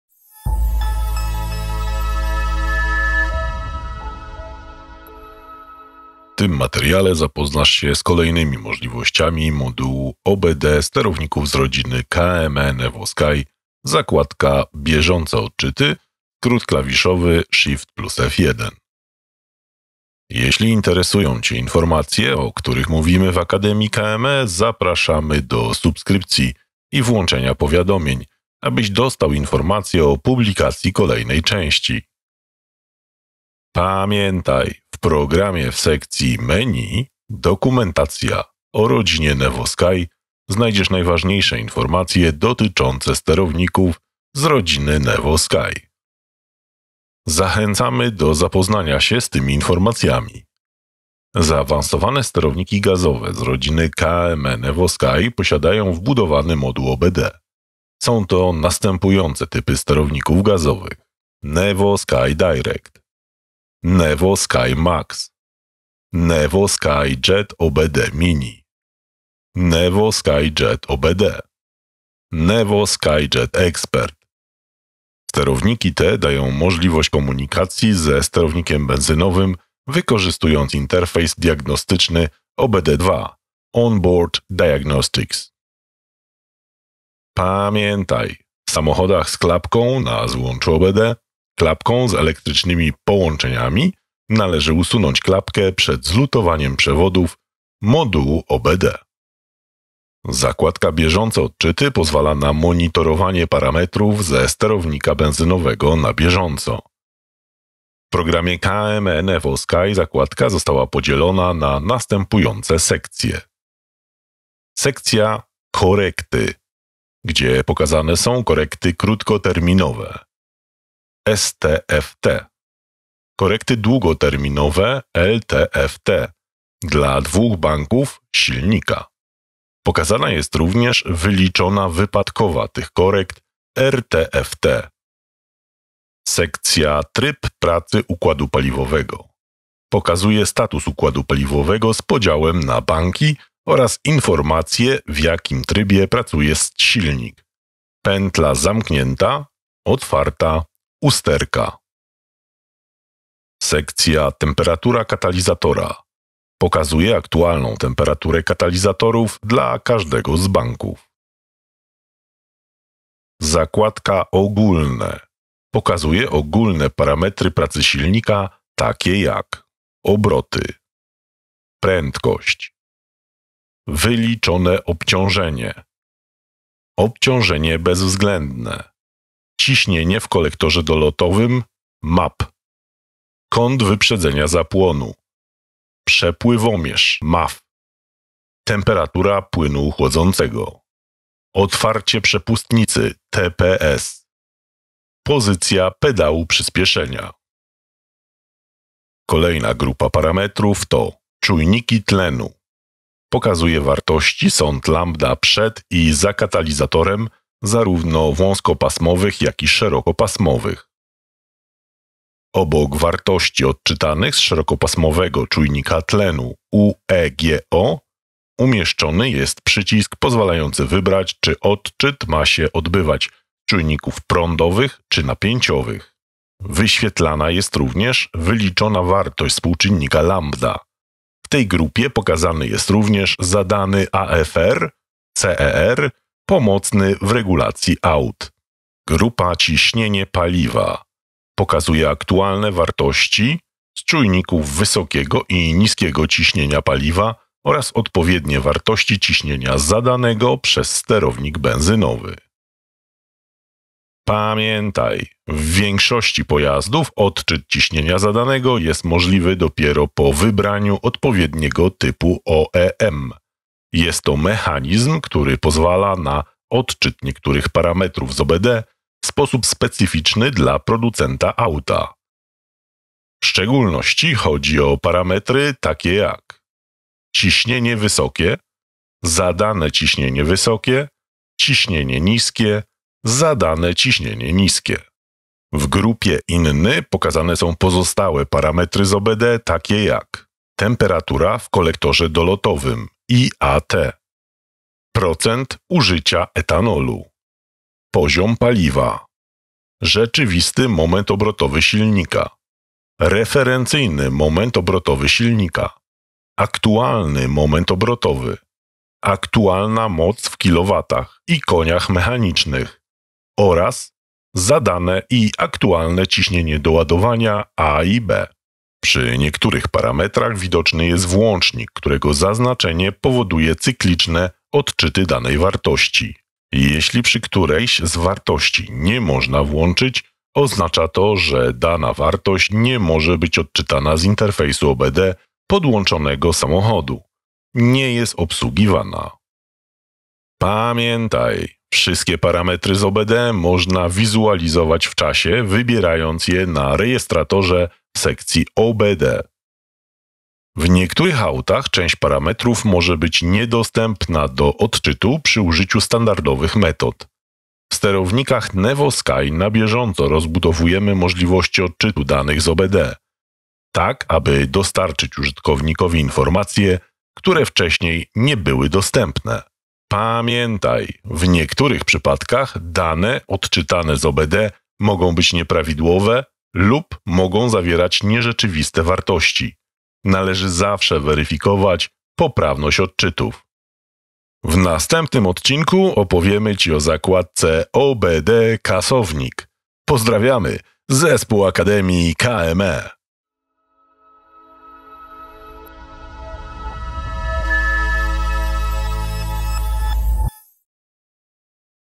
Moduł OBD w programie KME NEVO-SKY 5.0.7.5 cz3
W tym materiale zapoznasz się z kolejnymi możliwościami modułu OBD sterowników z rodziny KME NEVO-SKY zakładka Bieżące odczyty – skrót klawiszowy Shft+F1.

Zaawansowane sterowniki gazowe z rodziny KME NEVO-SKY posiadają wbudowany moduł OBD. Są to następujące typy sterowników gazowych:
NEVO-SKY DIRECT
NEVO-SKY MAX
NEVO-SKY JET OBD MINI
NEVO-SKY JET OBD
NEVO-SKY JET EXPERT

Sterowniki te dają możliwość komunikacji ze sterownikiem benzynowym wykorzystując interfejs diagnostyczny OBDII (On-Board Diagnostics).

Pamiętaj!!! W samochodach z klapką na złączu OBD (klapką z elektrycznymi połączeniami) należy usunąć klapkę przed zlutowaniem przewodów modułu OBD.

W dzisiejszym filmie omówimy poszczególne sekcje w zakładce
- Korekty
- Sekcja tryb pracy układu paliwowego
- Temperatura katalizatora
- Ogólne
- Czujniki tlenu
- Ciśnienie paliwa
- Inny

W niektórych autach część parametrów może być niedostępna do odczytu przy użyciu standardowych metod. W sterownikach NEVO-SKY na bieżąco rozbudowujemy możliwości odczytu danych z OBD, tak aby dostarczyć użytkownikowi informacje, które wcześniej nie były dostępne.
Pamiętaj!!! W niektórych przypadkach dane odczytane z OBD mogą być nieprawidłowe lub mogą zawierać nierzeczywiste wartości. Należy zawsze weryfikować poprawność odczytów.